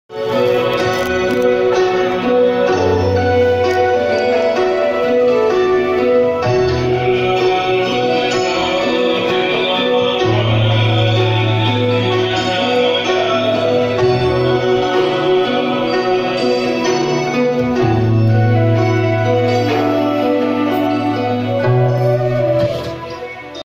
Music Concert in Saint peter burg Russia
see the Russian songs in the music concert in saint peter burg last year's.